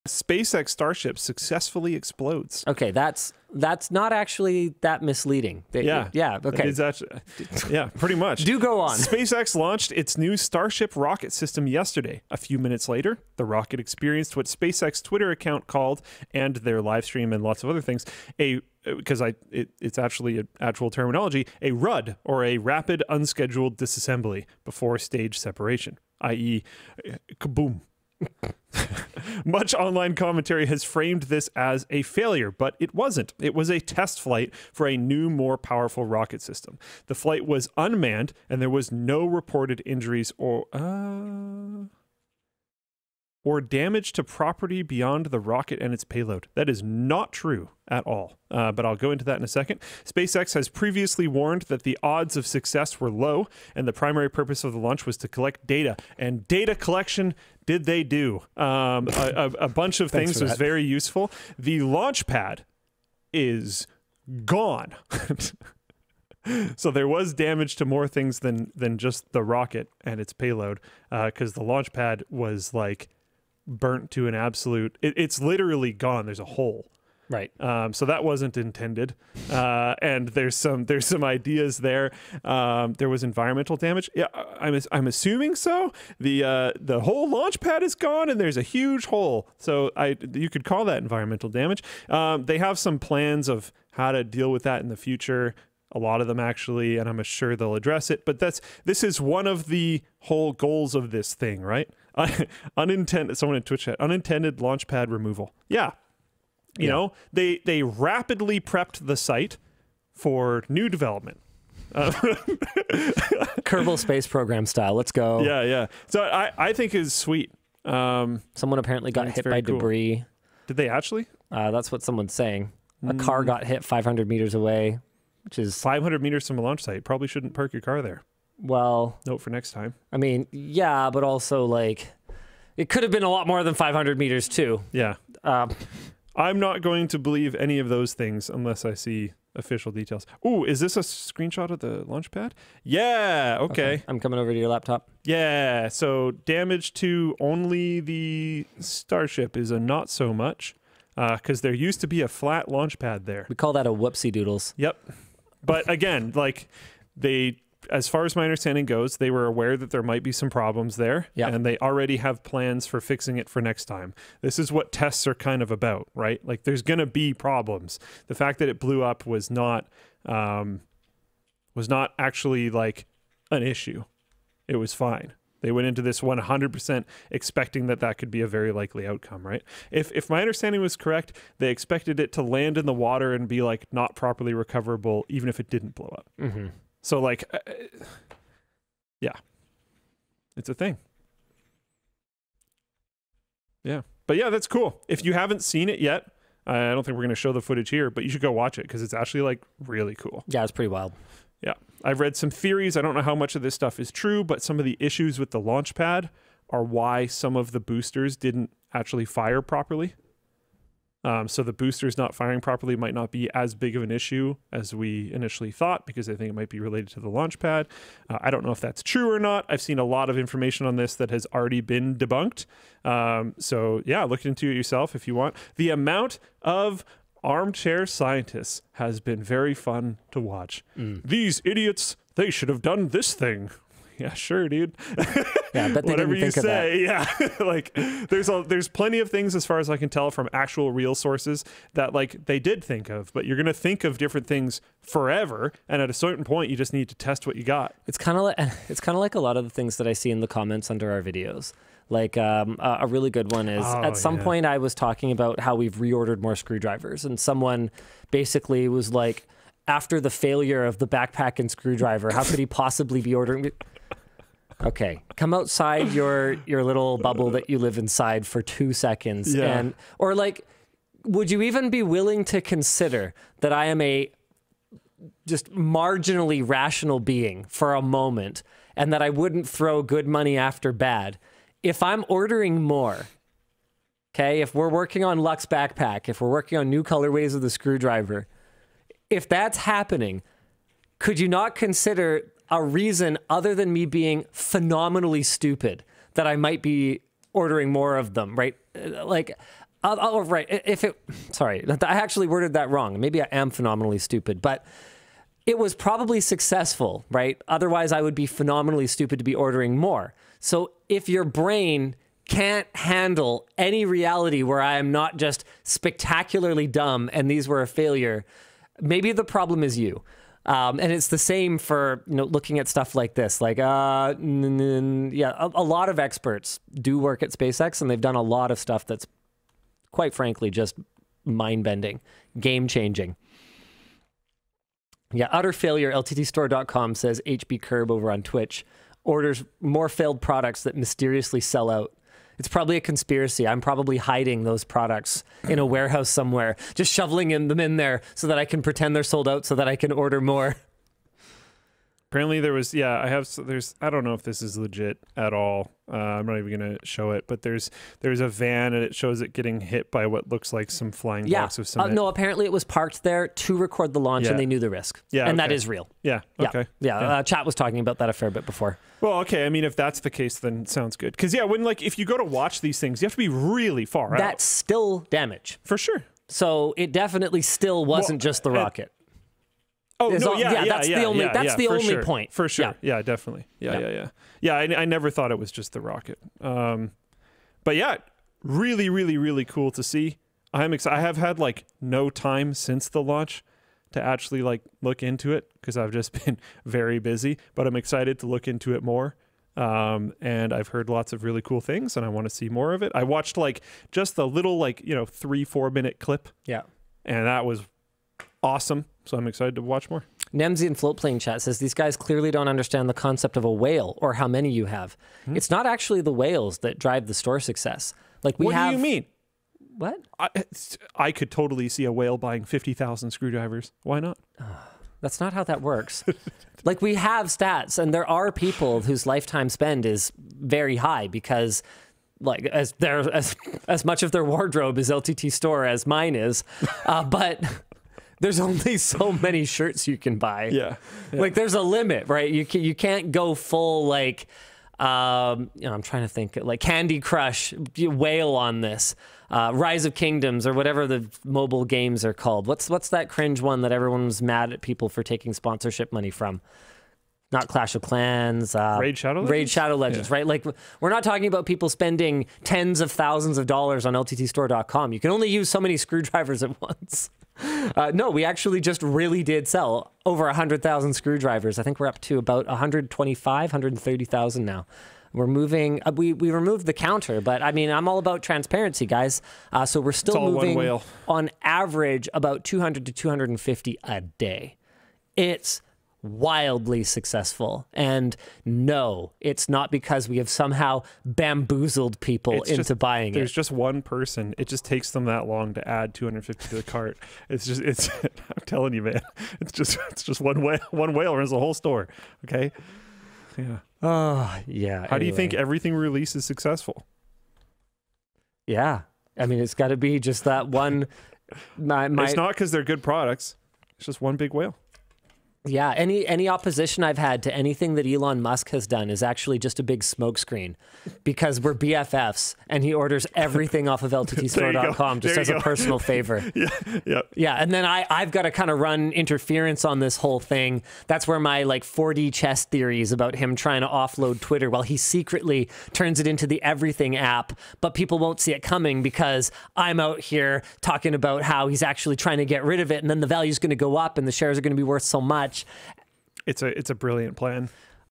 SpaceX Rocket Successfully Explodes
SpaceX test launched their new Starship rocket system.